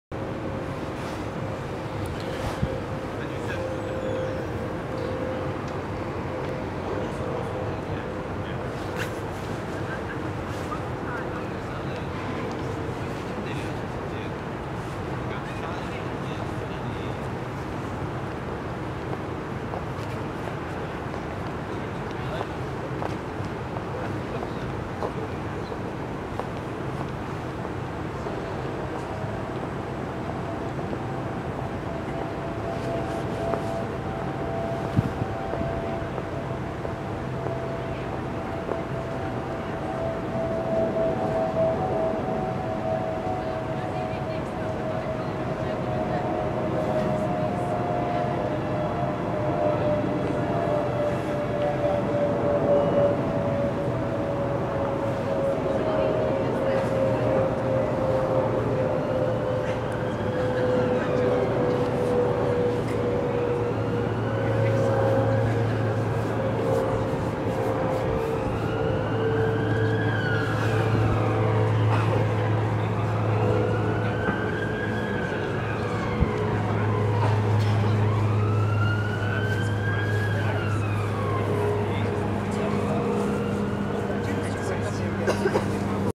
St Katherines Dock London at Night
Night view of londons famous st katherines dock